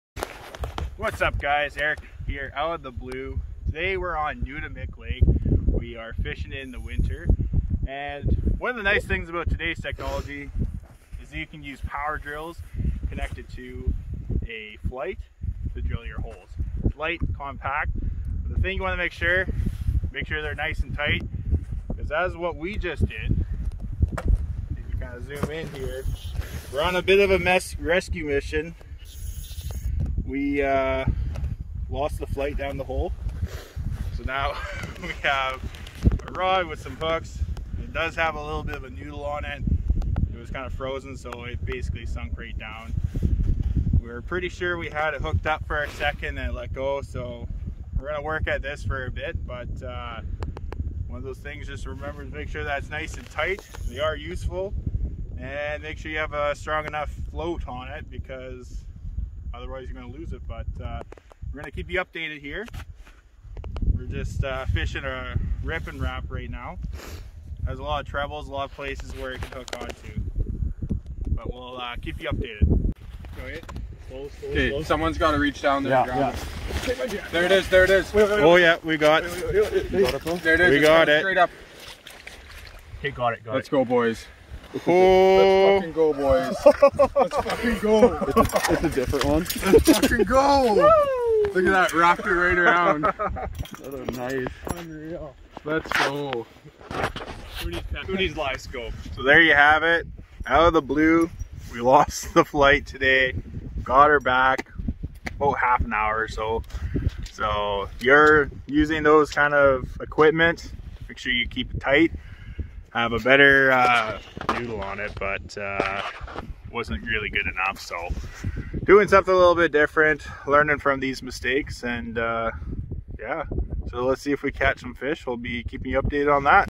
Auger Flyte Rescue Misson
Little late to post but this winter we lost our auger flyte on our power drill setup. We were able to rescue the flyte with some heavier tackle we had in the setup. Lesson to take away is make sure it's secure or better yet add a plate that prevents it from going through the hole.